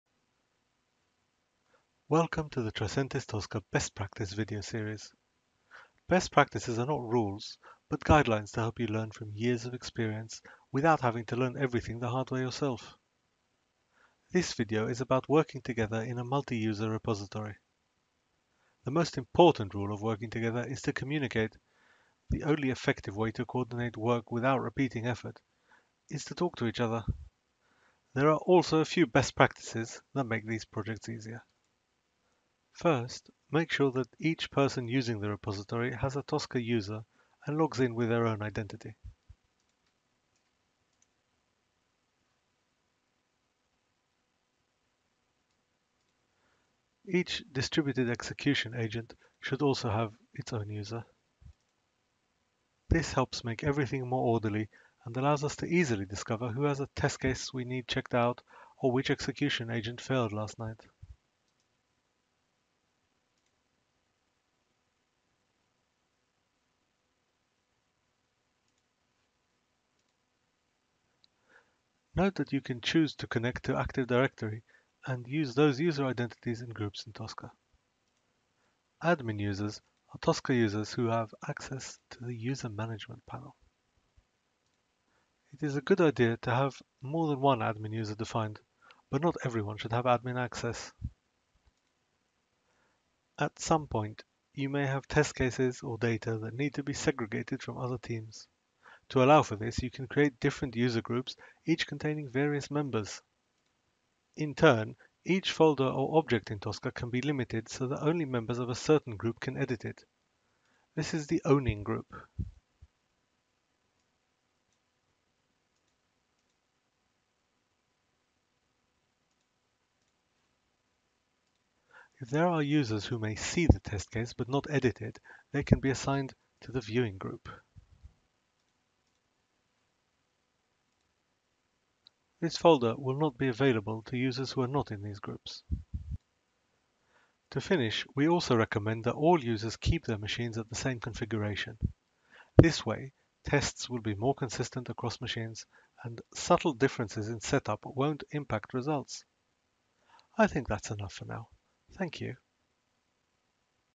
Tosca Best Practices: User Organization
In this video, you will learn tips for effective organization when working in a multi user environment. Carmi shows you the best ways to structure and assign user groups.